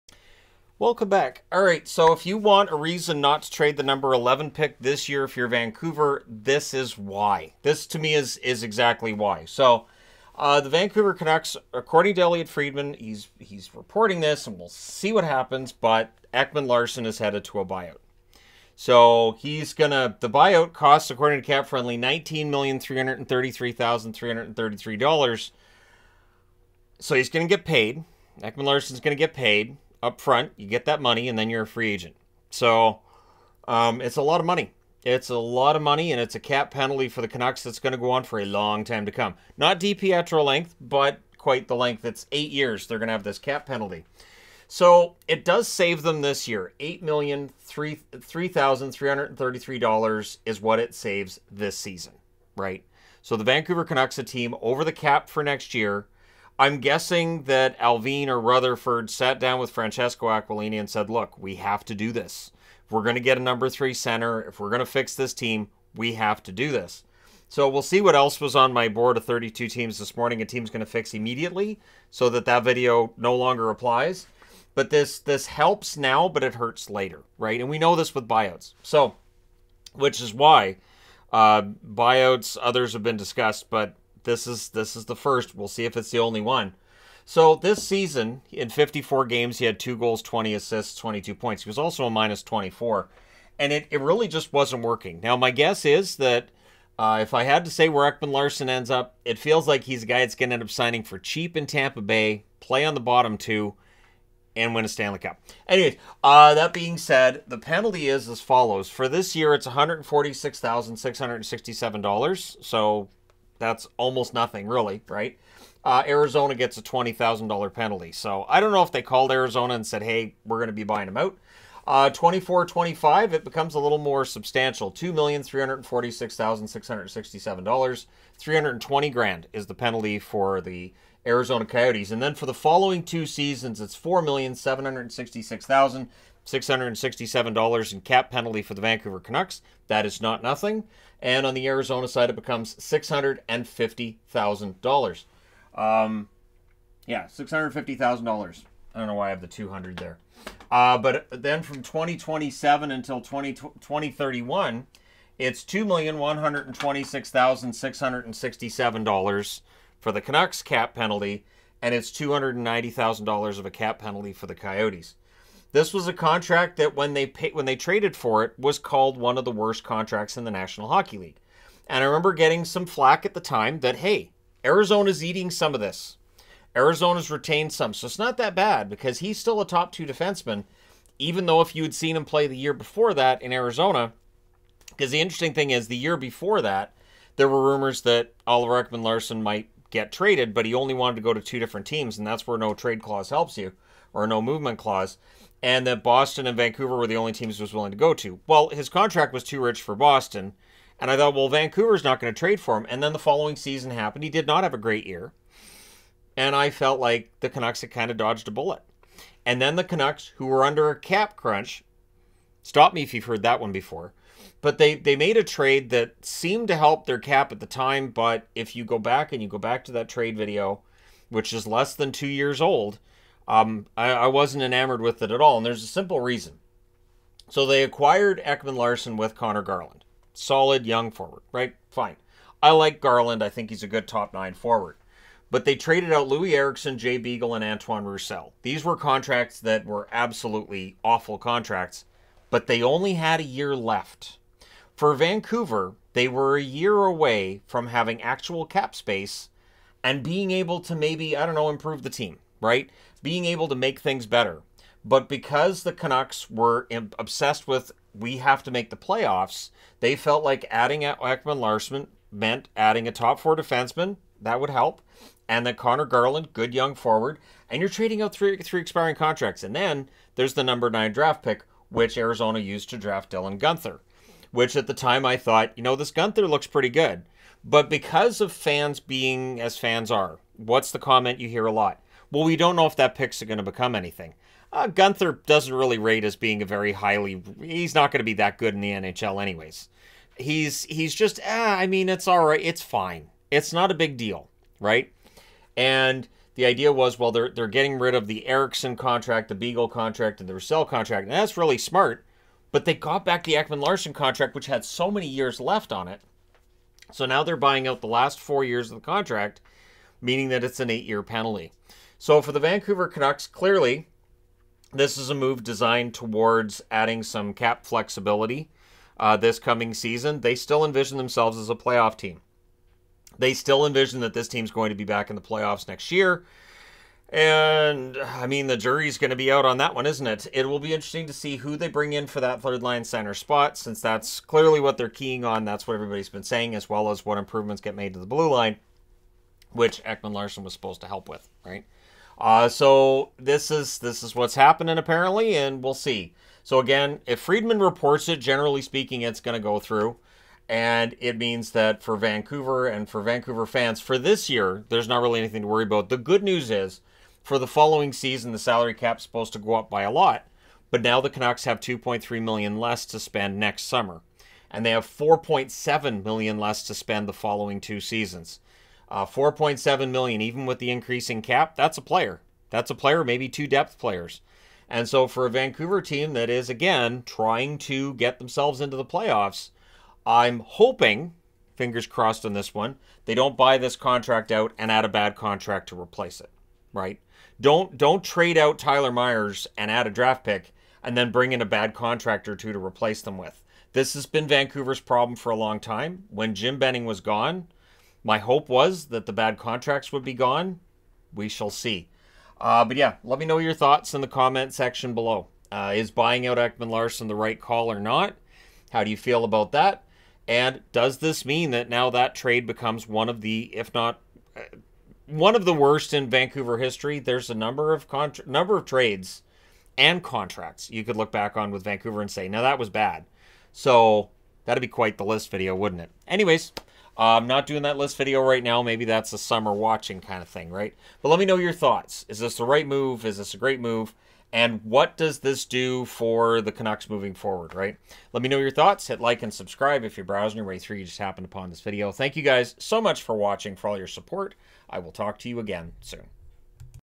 Canucks Reportedly Set to Buyout Ekman-Larsson
This is an expensive buyout and will affect the team for a long time to come.

Abbotsford, BC
V2S 8P1

In the USA:
Shannon Skanes
1125 Fir Avenue
Suite 119